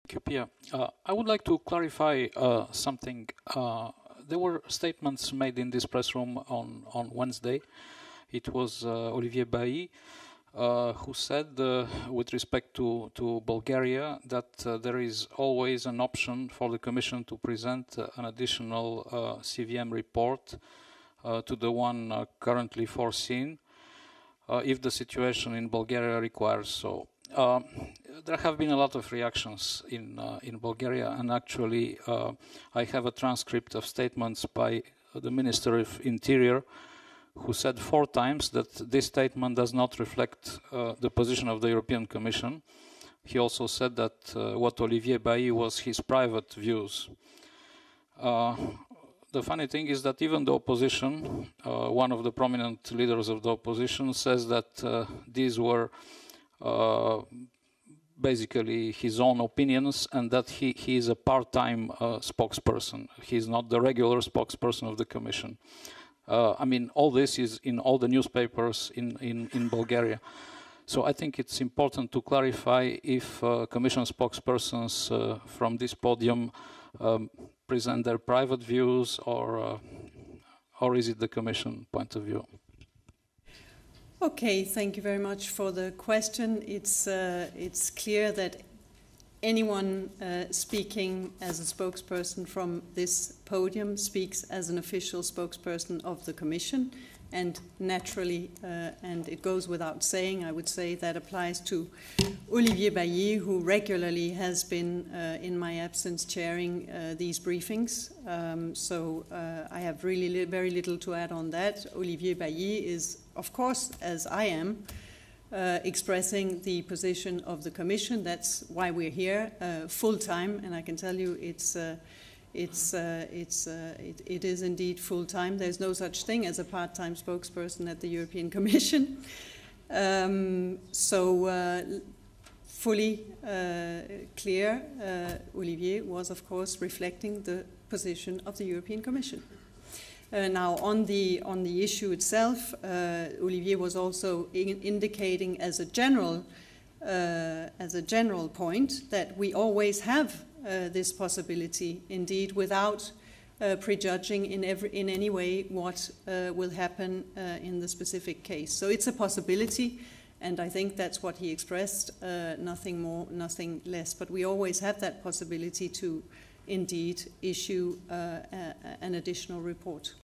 Хансен: Разбира се, че е официална позиция на ЕК!
Изявлението, дадено миналата седмица от говорителя на Европейската комисия Оливие Байи, за възможен допълнителен мониторингов доклад за България, отразява официалната позиция на Еврокомисията. Това заяви в Брюксел говорителят на председателя Жозе Барозу Пия Хансен. Тя отрече колегата й да е временен говорител или да е изразявал лично мнение.

"Разбира се, че всеки от нас, говорителите, когато излезе тук на тази трибуна, това е официална позиция на Комисията. Ние всички работим тук на пълен работен ден и няма такова нещо като работа на половин работен ден", - смеейки се заяви г-жа Хансен

Коментарът на Байи дойде след избора на Венета Марковска за конституционен съдия. Вътрешният министър заяви, че изявлението на Байи не било официална позиция, а министър-председателят Бойко Борисов настоя да разбере какви точно са обвиненията на Европейската комисия.